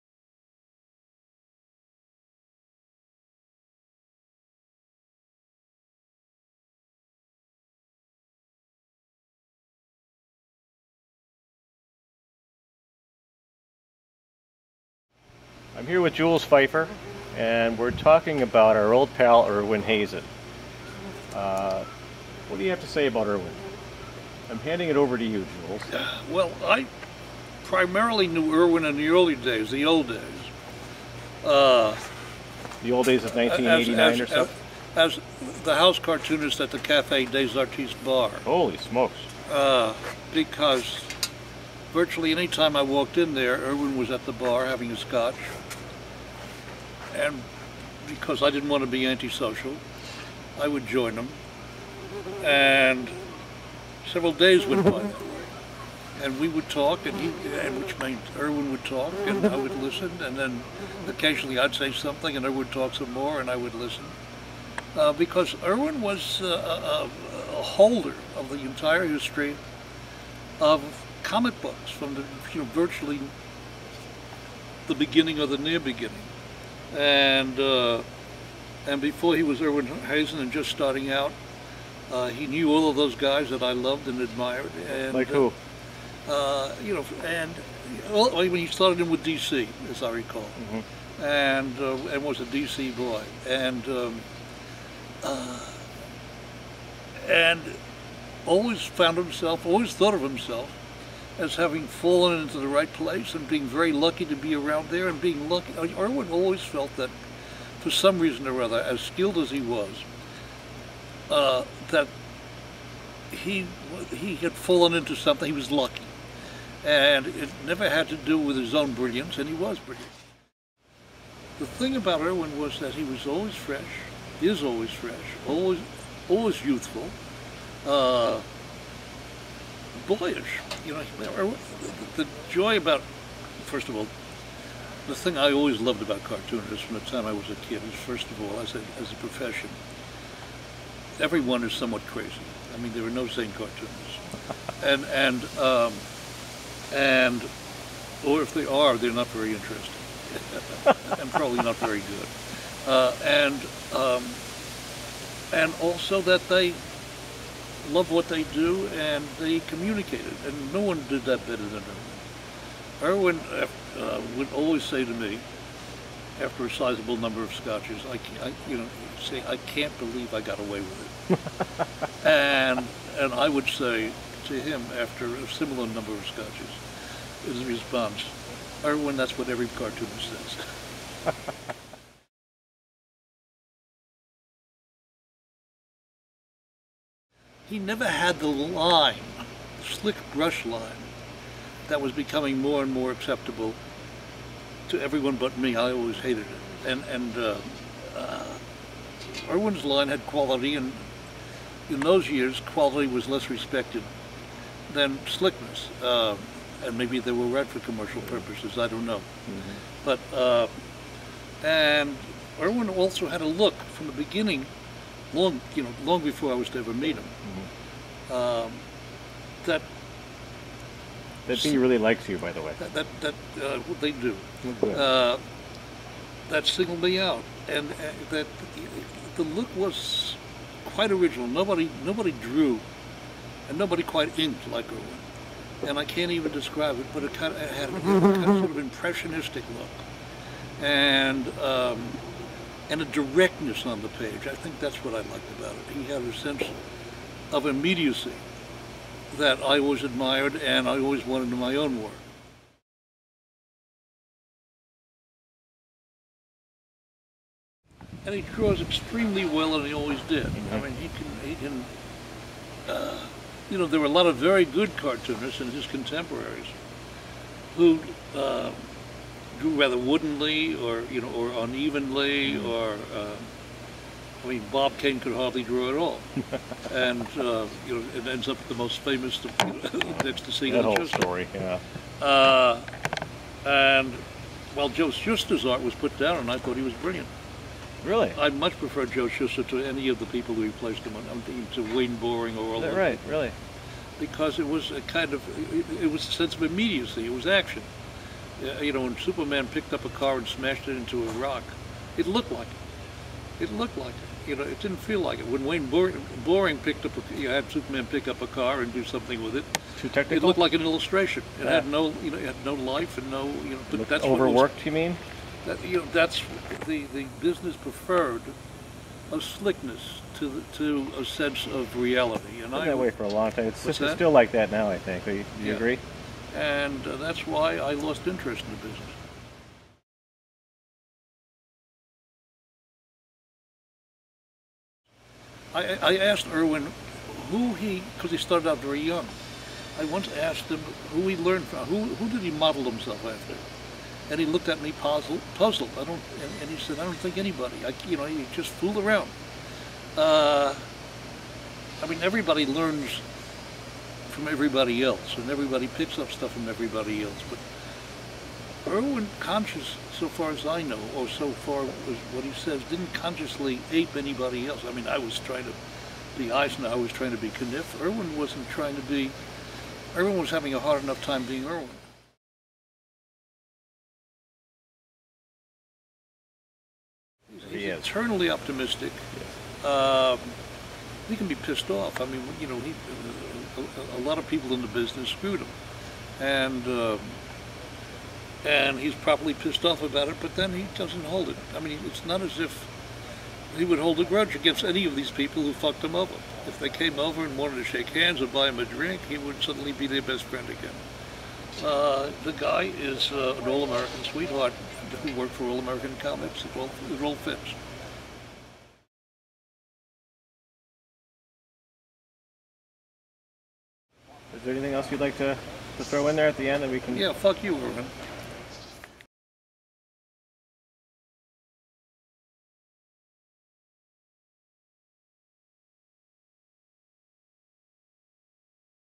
An interview with the great Jules Feiffer, talking about cartoonist Irwin Hasen
A number of years ago, the Manhattan chapter of the National Cartoonists Society honored cartoonist Irwin 'Dondi' Hasen at the Society of Illustrators in NYC.  Longtime friend Jules couldn't make it, so we went out to his place and had him share some stories about Irwin.